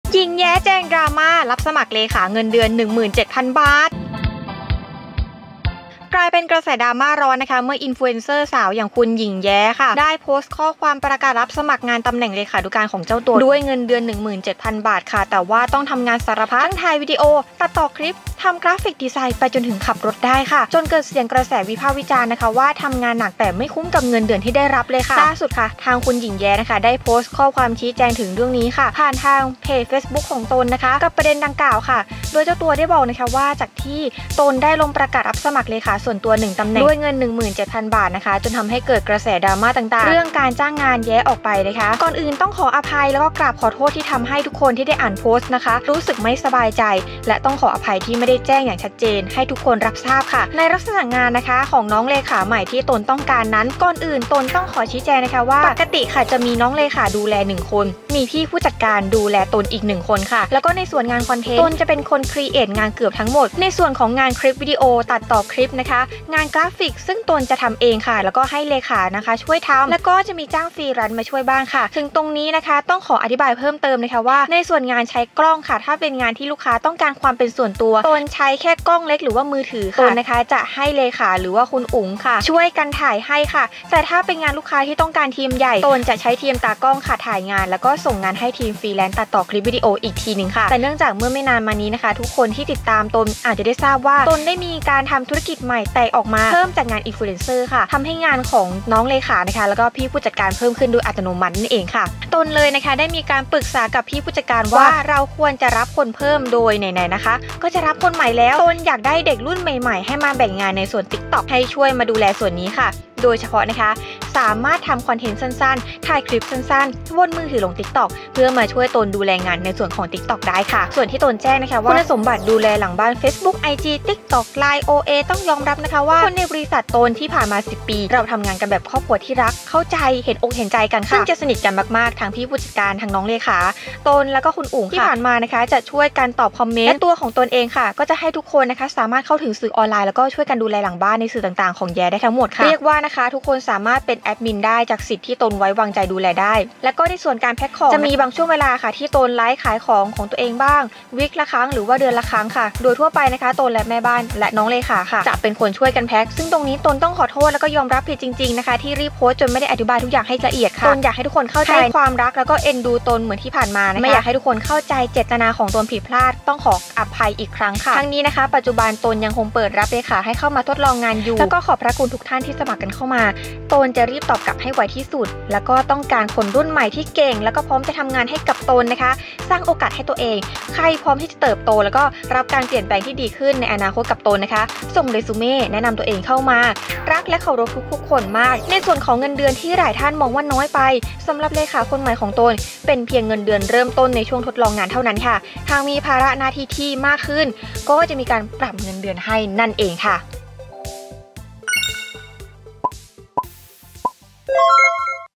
หญิงแย้ แจงดราม่า รับสมัครเลขาฯ เงินเดือน 17,000 บาท I mayahotnews
หญิงแย้ แจงดราม่า รับสมัครเลขาฯ เงินเดือน 17,000 บาท